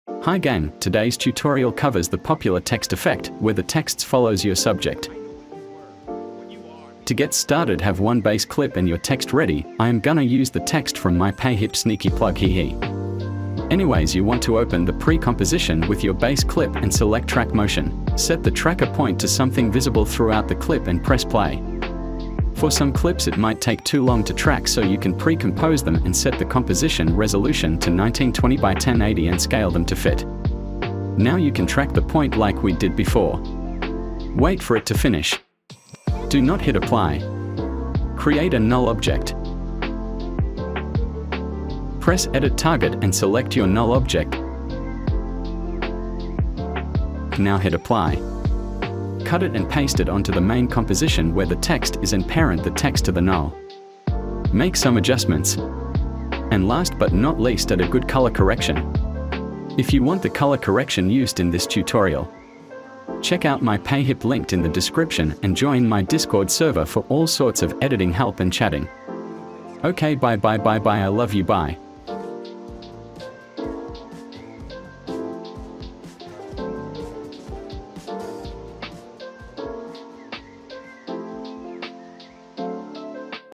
Track Text To Face *TUTORIAL*
this video covers the facetrack text effect that is being used by famous editors these days on Tiktok / Reels / Shorts.
Socials :
make sure you guys like and sub for more :)!
Similar search keywords:
facetrack text after effects
how to make text follow face tutorial
text following face tutorial
after effects popular text animation.